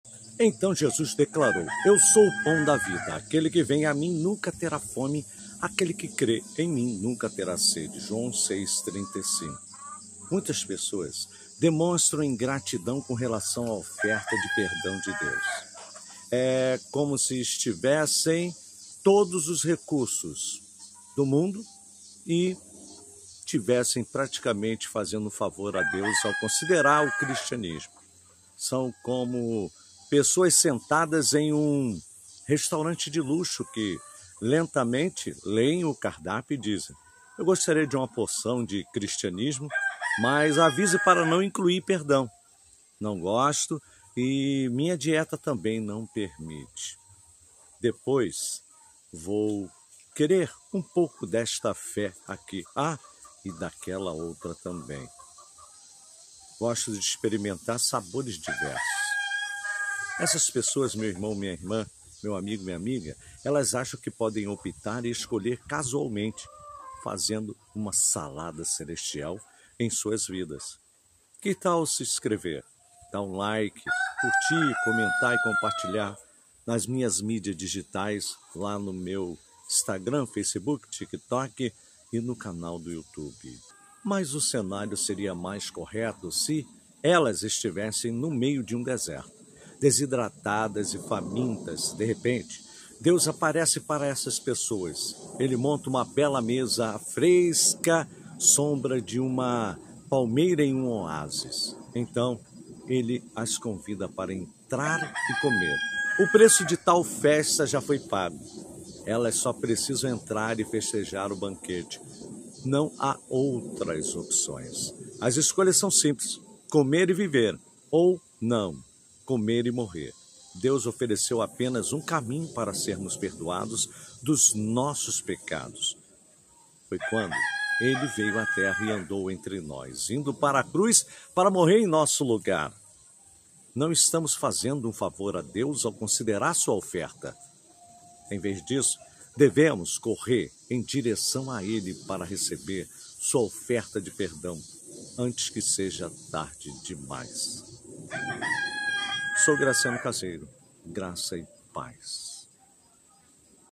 SALADA CELESTIAL
"Então Jesus declarou: 'Eu sou o pão da vida. Aquele que vem a mim nunca terá fome; aquele que crê em mim nunca terá sede.'" (João 6:35)

Muitas pessoas demonstram ingratidão com relação à oferta de perdão de Deus. É como se tivessem todos os recursos do mundo e estivessem praticamente fazendo um favor a Deus ao considerar o cristianismo.
São como pessoas sentadas em um restaurante de luxo que lentamente leem o cardápio e dizem: "Eu gostaria de uma porção de Cristianismo, mas avise para não incluir perdão. Não gosto, e minha dieta também não permite. Depois vou querer um pouco desta fé aqui. Ah, e daquela outra também. Gosto de experimentar sabores diversos." Elas acham que podem optar e escolher casualmente, fazendo uma "salada celestial" em suas vidas. Que tal se inscrever? Dá um like. Curtir. Comentar e compartilhar.
Mas o cenário seria mais correto se elas estivessem no meio de um deserto, desidratadas e famintas. De repente, Deus aparece para essas pessoas. Ele monta uma bela mesa, à fresca sombra de uma palmeira em um oásis. Então, Ele as convida para entrar e comer. O preço de tal festa já foi pago. Elas só precisam entrar e festejar o banquete. Não há outras opções. As escolhas são simples: comer e viver, ou não comer e morrer. Deus ofereceu apenas um caminho para sermos perdoados dos nossos pecados. Foi quando Ele veio à terra e andou entre nós, indo para a cruz para morrer em nosso lugar. Não estamos fazendo um favor a Deus ao considerar Sua oferta. Em vez disso, devemos correr em direção a Ele para receber Sua oferta de perdão, antes que seja tarde demais. Sou Graciano Caseiro. Graça e paz.